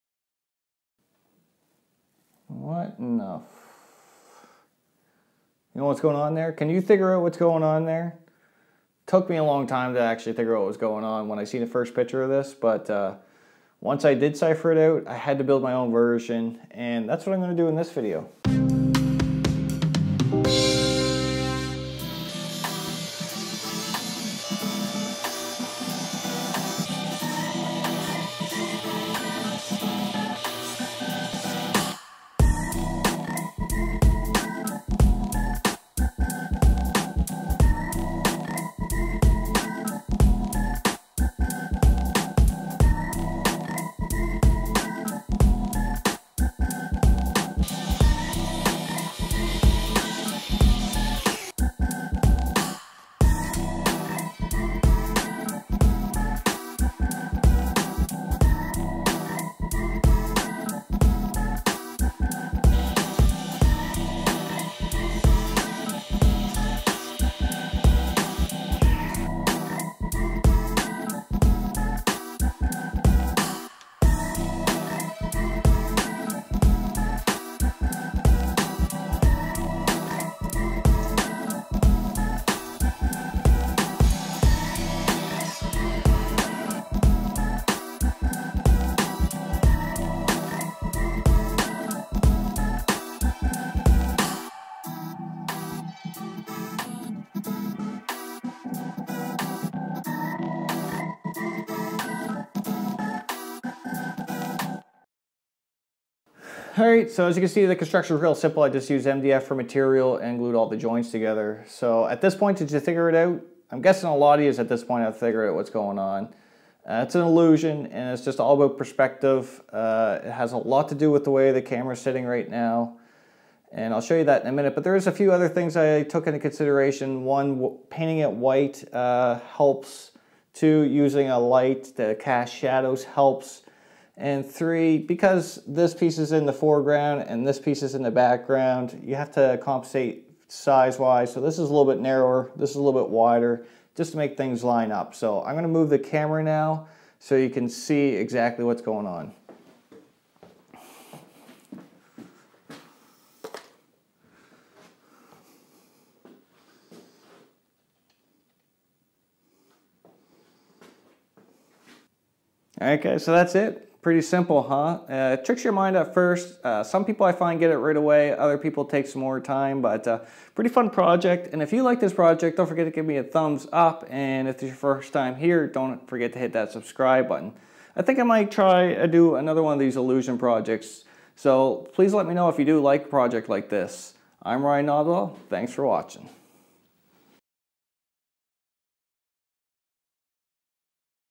Impossible or illusion??
I came across this illusion on the internet and it really intrigued me. I actually couldn't find a solution to how it was done anywhere and the picture was really well done so i'm still not 100 percent sure if this is how it was made. I came up with what i thought was the solution to the illusion and even though its not perfect i think it will still fool some people.